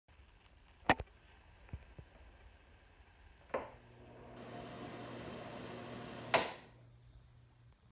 Broan bathroom fan
This is in the apartment bathroom. This is the exact same one that is in the main floor bathroom except that this one is actually quiet lol. Enjoy. Also this is the last bathroom fan at the new house.